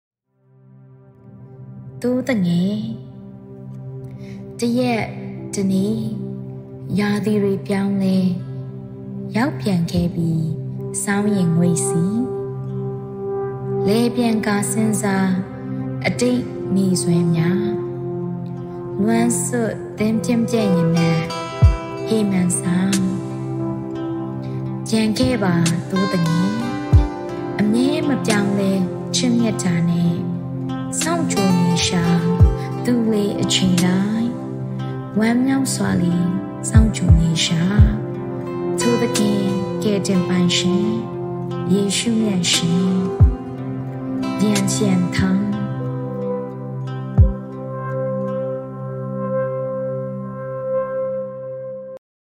သိုးသူငယ် - Lian Sian Thang | ရွတ်ဖတ်သူ - Cer | Myanmar Christian Audio Book
သိုးသူငယ်

တစ်ရက် တစ်နေ့
ရာသီတွေပြောင်းလဲ
ရောက်ပြန်ခဲ့ပြီ
ဆောင်းရင်ငွေ့ဆီ
လည်ပြန်ကာ စဥ်းစား
အတိတ်နေ့စွဲများ
လွမ်းဆွတ် သင်းပျံ့ပျံ့ရနံ့
ဟေမာန်ဆောင်း ...
ပြန်ခဲ့ပါ သိုးသူငယ်
အမြဲ မပြောင်းလဲ ချစ်မေတ္တာနဲ့
စောင့်ကြိုနေရှာ သူလေအချိန်တိုင်း
ဝမ်းမြောက်စွာလေ စောင့်ကြိုနေရှာ
ထိုသခင် ကယ်တင်ပိုင်ရှင် ယေရှုမြတ်ရှင်...

Lian Sian Thang

Audio Engineer - Chris Mang